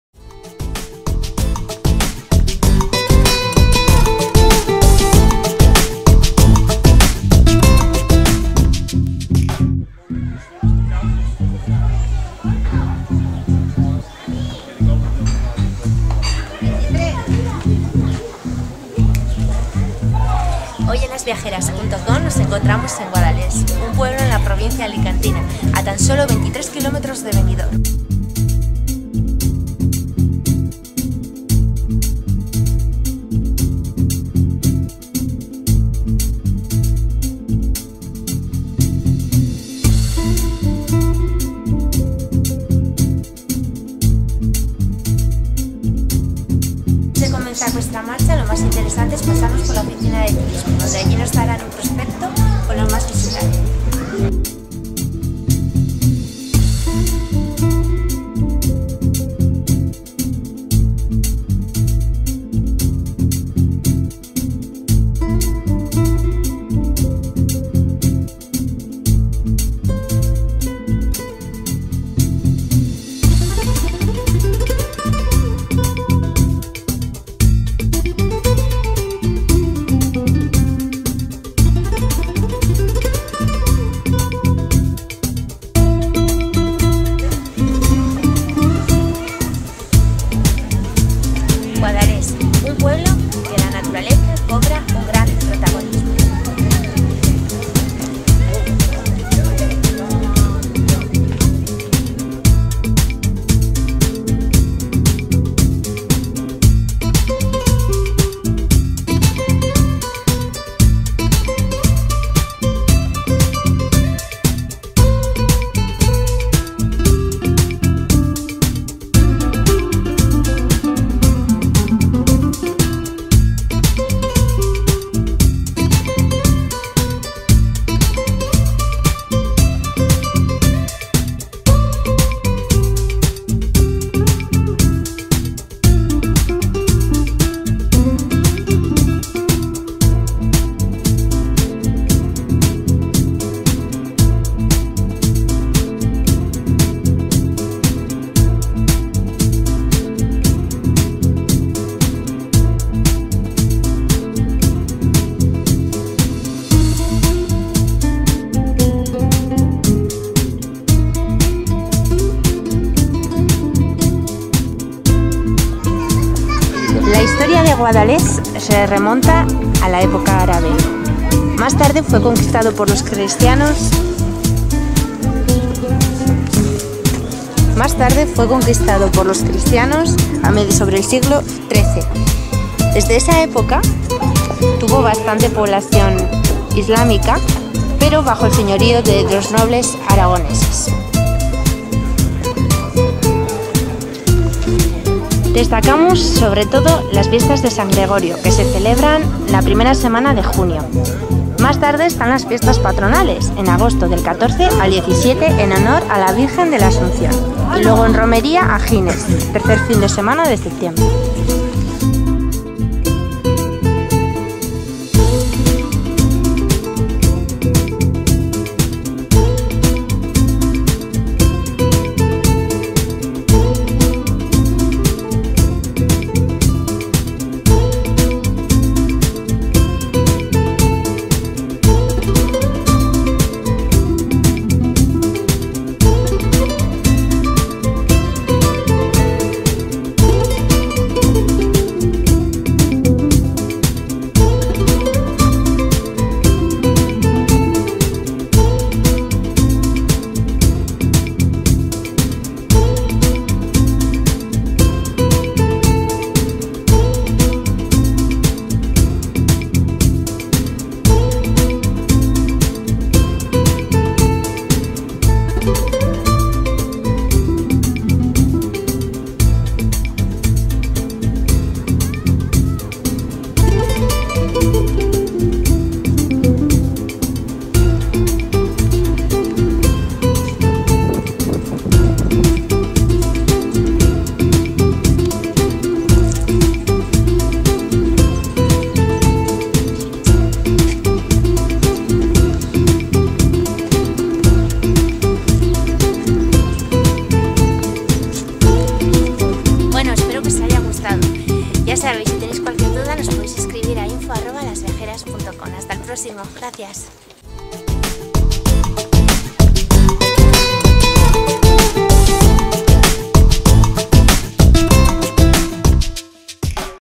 Guadalest
En este video lasviajeras.com hemos estado en Guadalest. Un pueblo pintoresco en la provicina de Alicante.